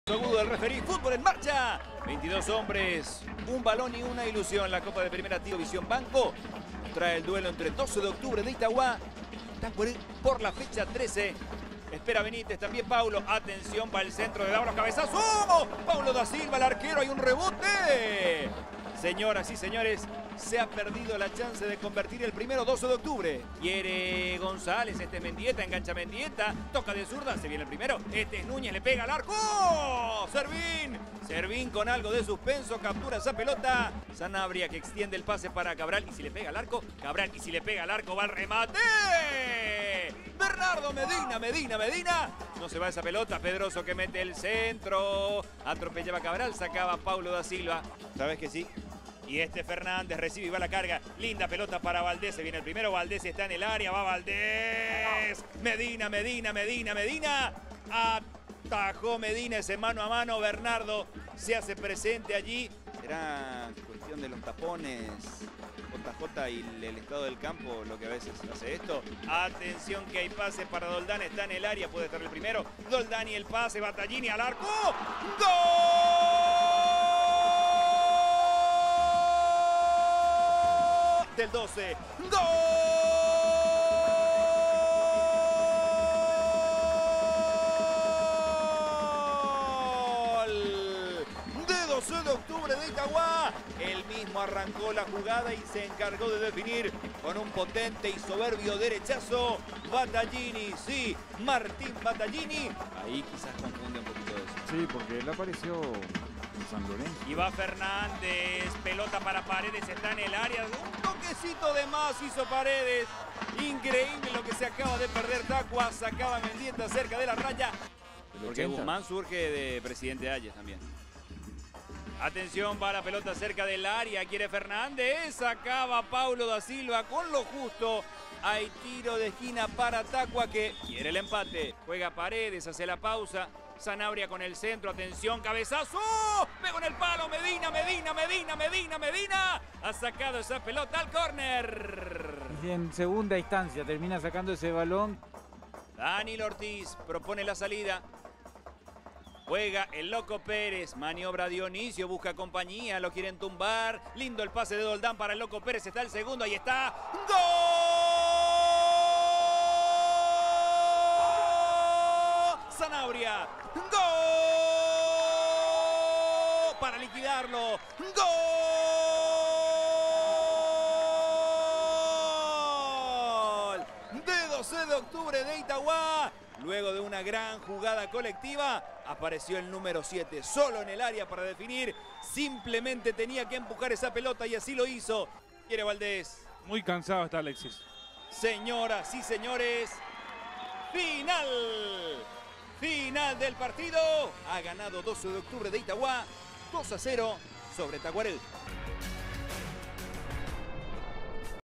12 de Octubre 2 - 0 Tacuary (TORNEO APERTURA 2022) Fecha 13
Goles: ⚽️ 54’ M. Batallini (12)
93’ N. Sanabria (12)
Video: Tigo Sports PY.